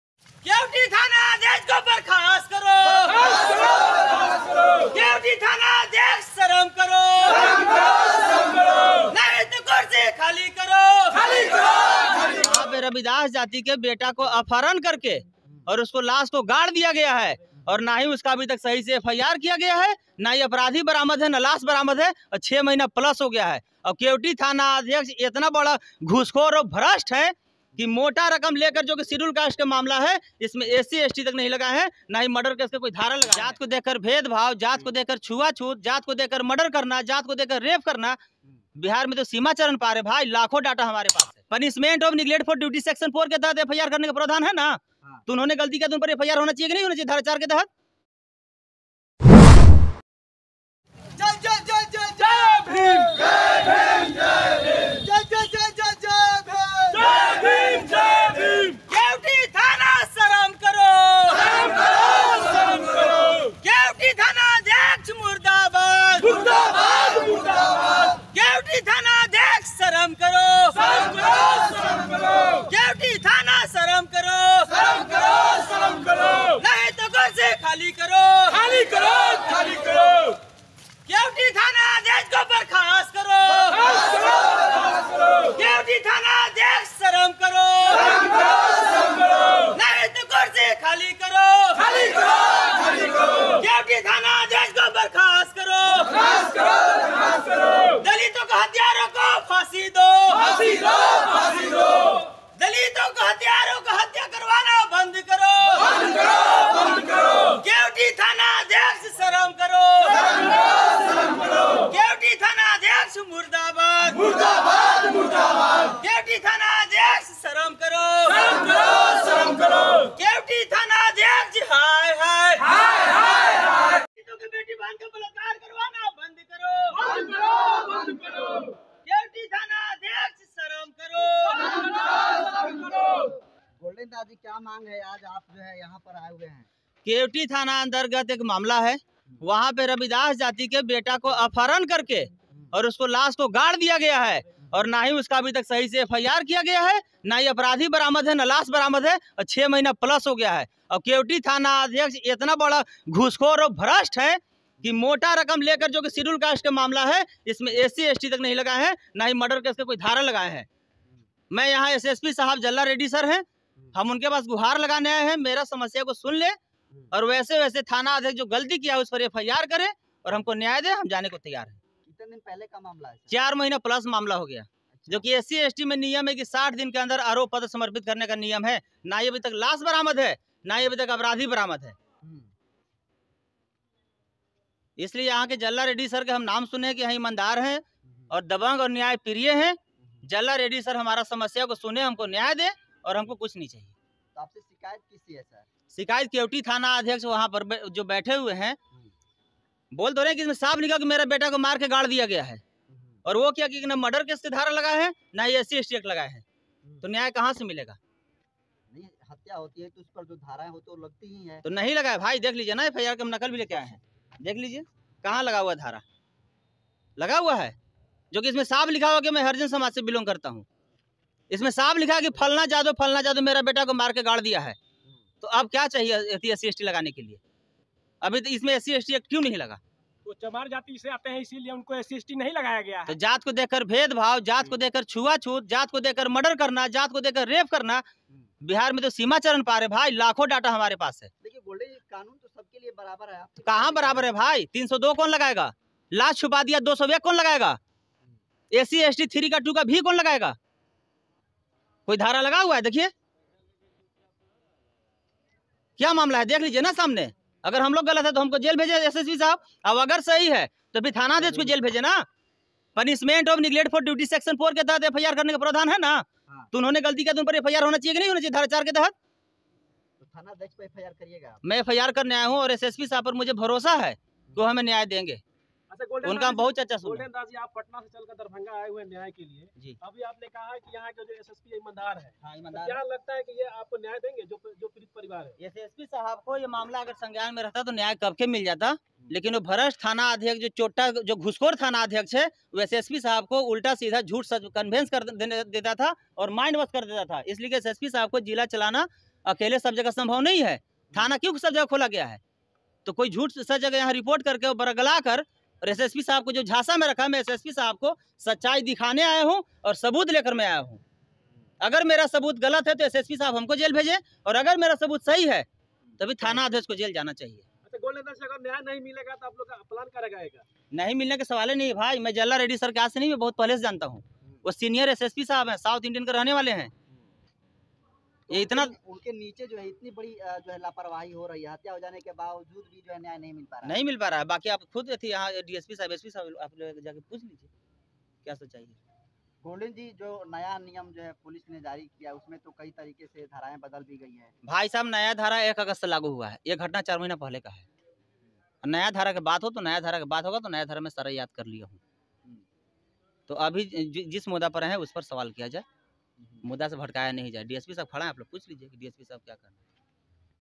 हजारों समर्थकों के साथ थाना में ताला मारने पहुँचे Golden Das को देख पुलिस वालों के छूटे पसीने ।।
हजारों समर्थकों के साथ थाना में ताला मारने पहुँचे Golden Das को देख पुलिस वालों के छूटे पसीने ।। Jantantra khabar
golden das ka thana me tala
golden das darbhanga news
golden das Jantantra khabar
golden das ka video